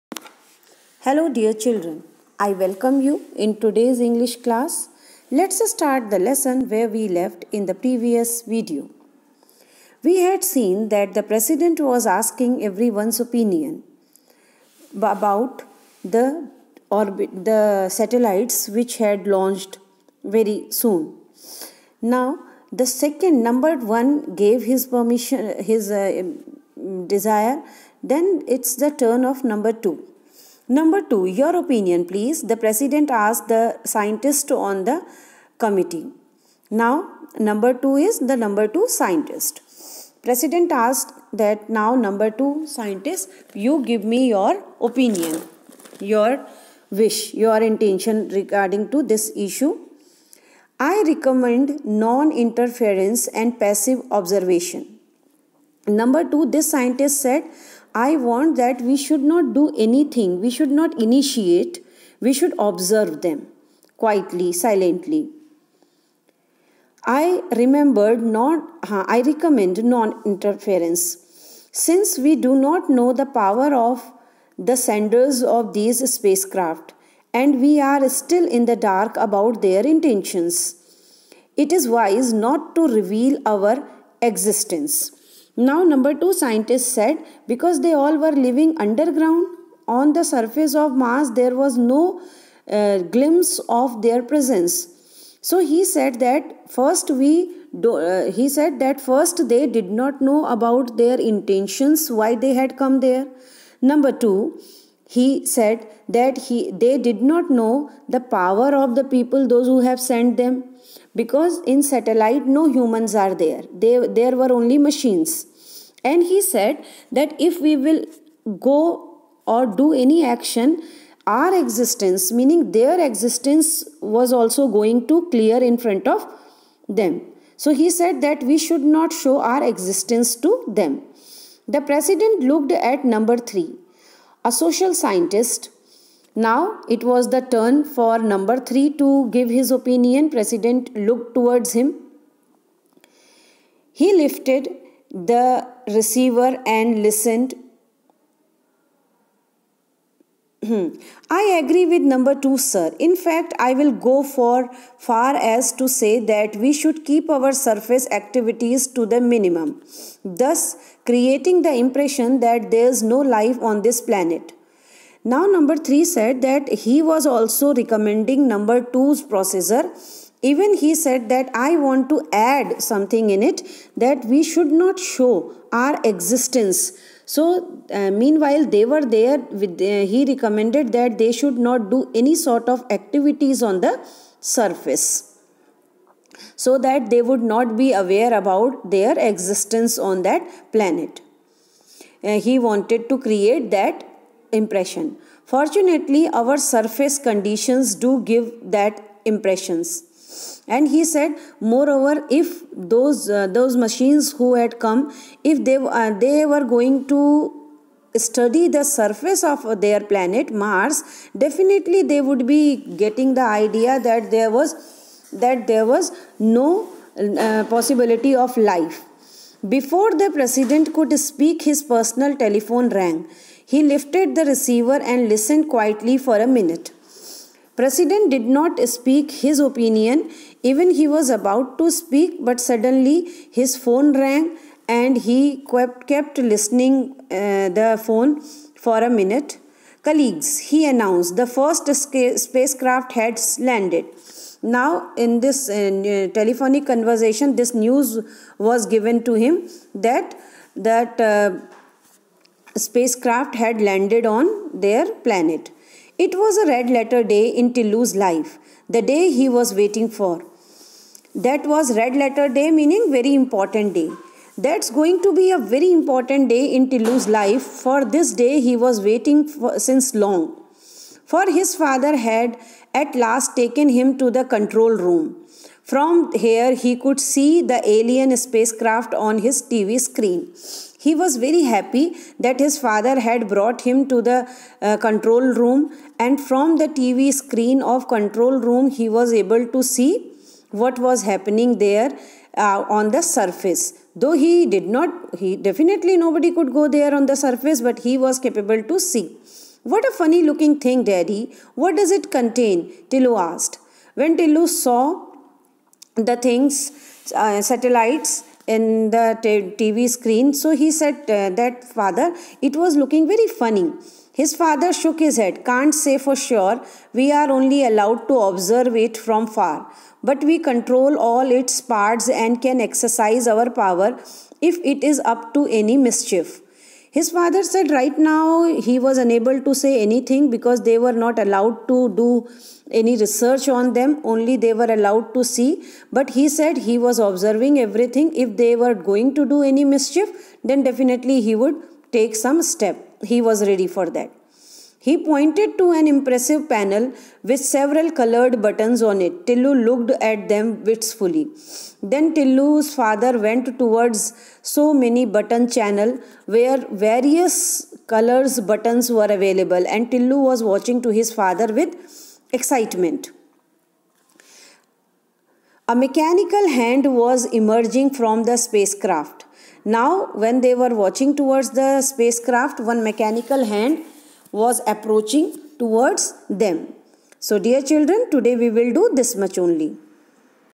Lesson. An Alien Hand ( video 5) Subject- English Class. VII By Dr. Beena Karnatak TGT English KV1
Video from Khimesh Chandra Bhatt